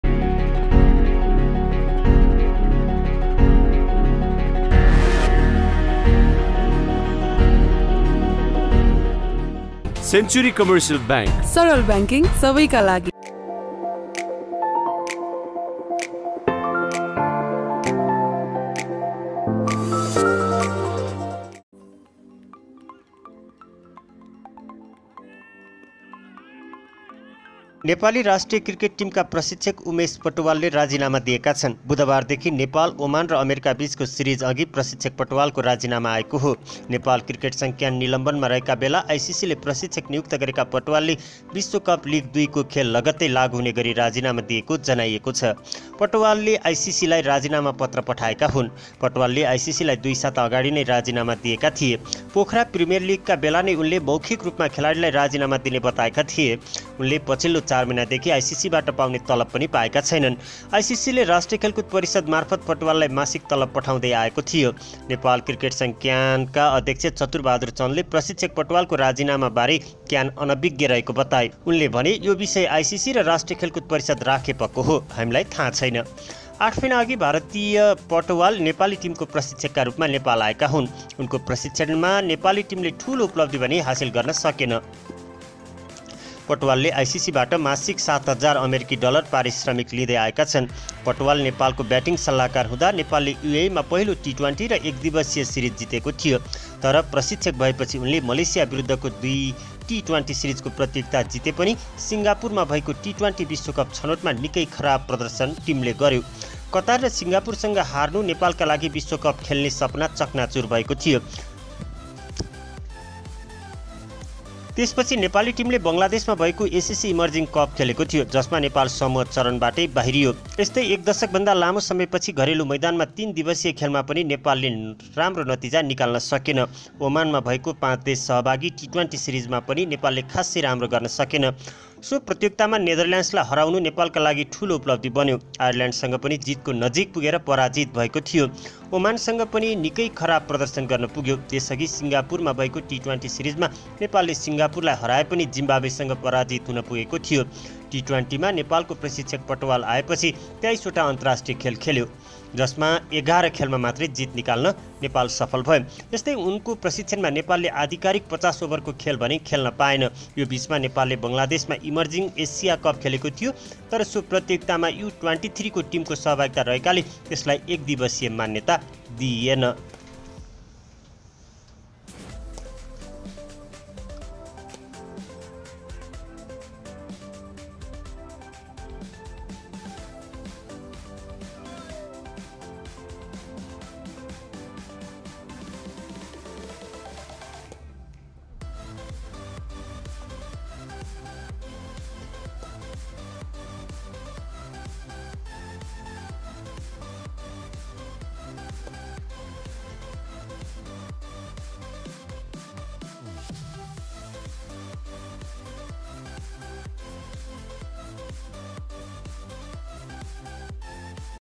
Why Umesh Patwal Resign from national Cricket Team | Nepal cricket team
नेपाली राष्ट्रिय क्रिकेट टिमका प्रशिक्षक उमेश पटवालले राजीनामा दिएका छन् । बुधबारदेखि नेपाल, ओमान र अमेरिकाबीचकाे सिरिज अघि प्रशिक्षक पटवालकाे राजीनामा आएकाे हाे । नेपाल क्रिकेट संघ क्यान निलम्बनमा रहेका बेला आईसीसीले प्रशिक्षक नियुक्त गरेका पटवालले विश्वकप लिग २ काे खेल लगत्तै लागूहुने गरि राजीनामा दिएकाे जनाइएकाे छ । पटवालले आईसीसीलाई राजीनामा पत्र पठाएका हुन् ।

Edit: Manoj Basnet
Camera : Amar Bhandari
Producer: Mukti Babu Regmi
Post Production: Neplays Media Management.